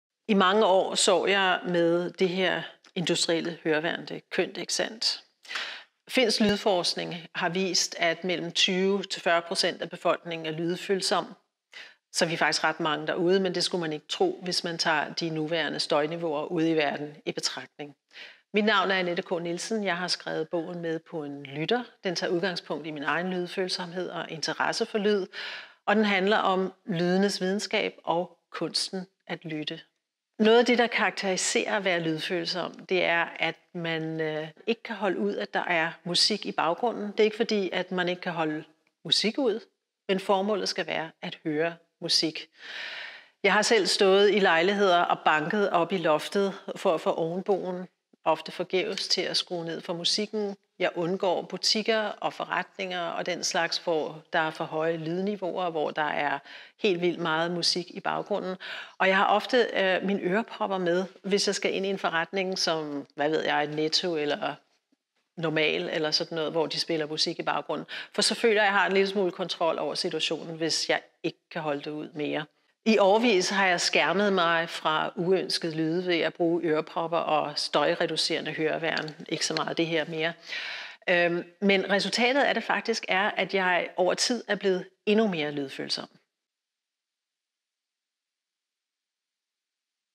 Annette K. Nielsen fortæller om bogen Med på en lytter
Med på en lytter er en personlig og perspektivrig bog om lyden af verden og menneskene i den.